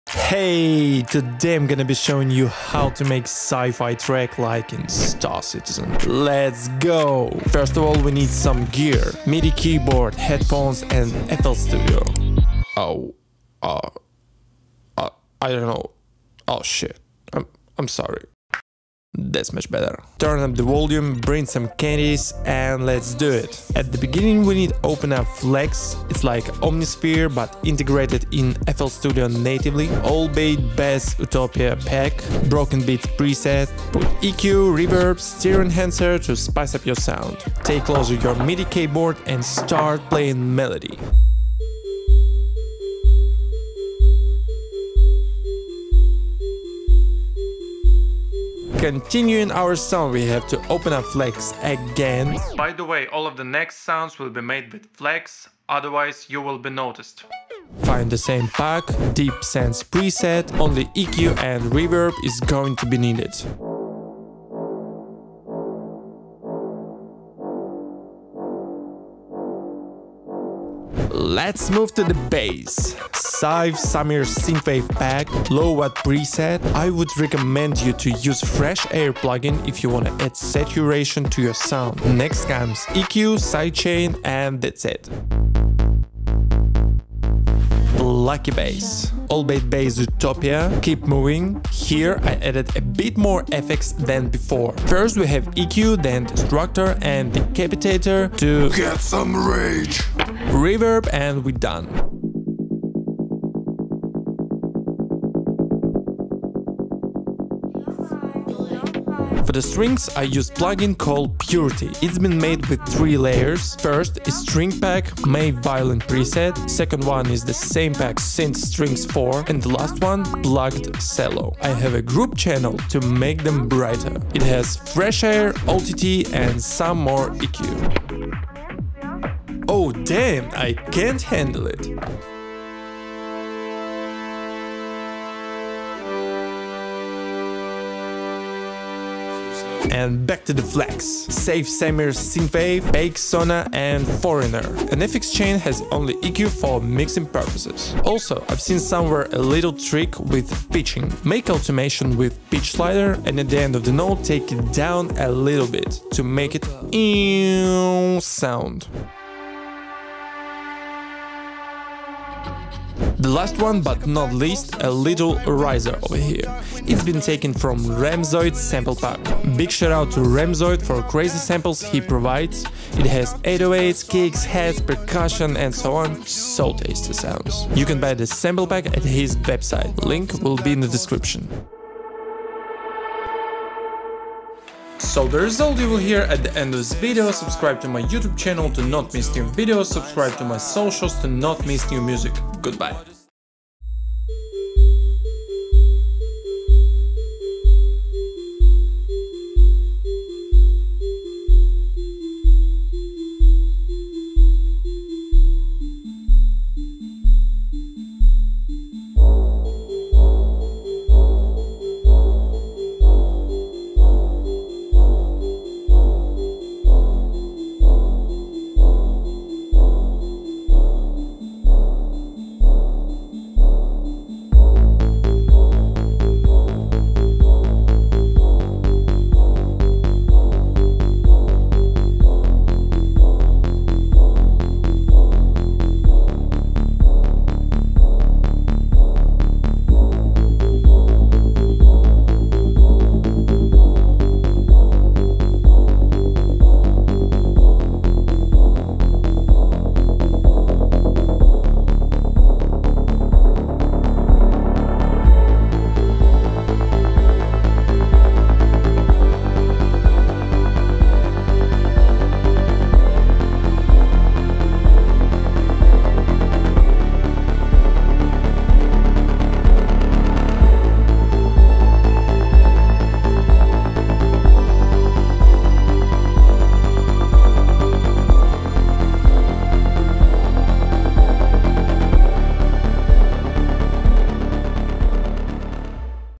How to make SCI-FI music
Hello ! Today I'm gonna show you how to make SCI - FI music like in space simulator game Star Citizen.

►Follow AndyKutson